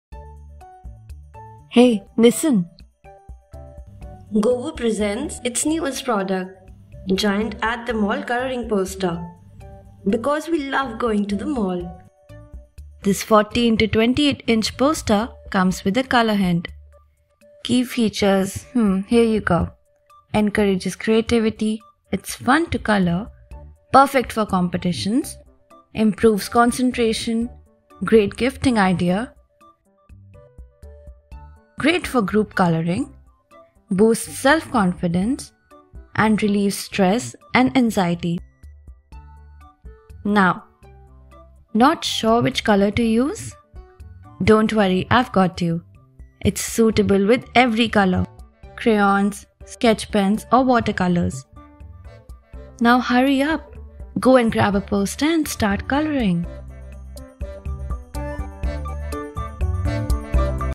Giant At The Mall Colouring Poster: fun, colorful, detailed, playful, enjoyable for all ages.
Welcome to our At the Mall Colouring Poster video!

In this video, we will be showcasing our fun-filled At the Mall Colouring Poster. This poster features a bustling shopping mall scene with all kinds of shops, from fashion boutiques to electronics stores and toy shops.

With its large size and intricate details, this poster is perfect for hours of colouring fun. You can use markers, coloured pencils, or even paint to bring this shopping mall scene to life.

This poster is great for solo colouring or as a group activity for birthday parties, school events, or family gatherings. And when you're finished colouring, you can proudly display your masterpiece on the wall for all to see.

So get ready for a day of shopping and let your creativity run wild with our At the Mall Colouring Poster!